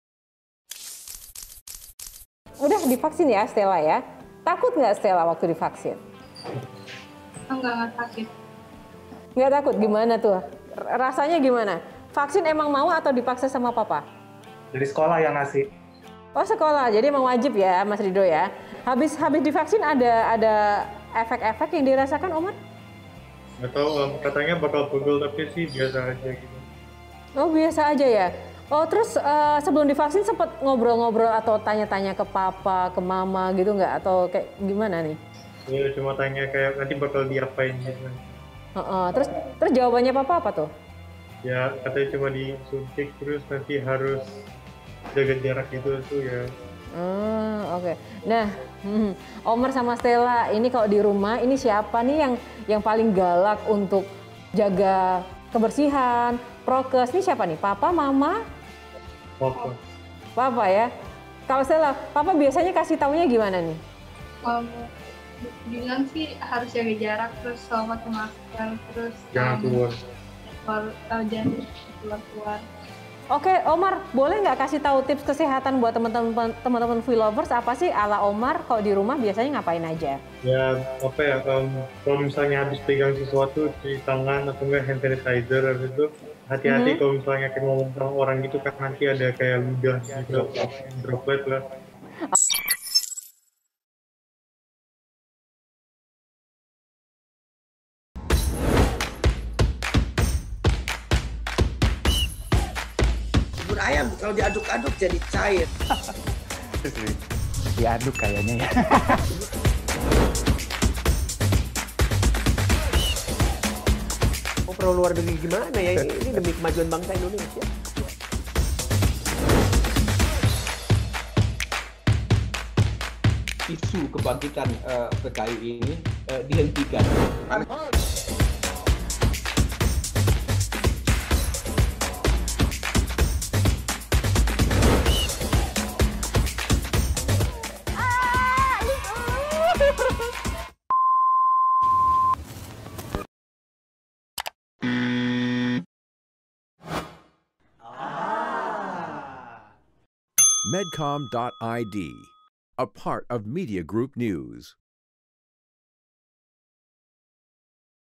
Vaksin Slank untuk Indonesia - Kesan Anak Ridho Slank usai Disuntik Vaksin COVID-19
Pemerintah telah memberlakukan program vaksinasi kepada anak yang berusia 12-17 tahun sejak 1 Juli 2021. Salah satu anak yang telah divaksin ialah anak Ridho Slank. Dalam kesempatan kali ini, kedua anak Ridho pun berbagi cerita bagaimana kondisi usai divaksin COVID-19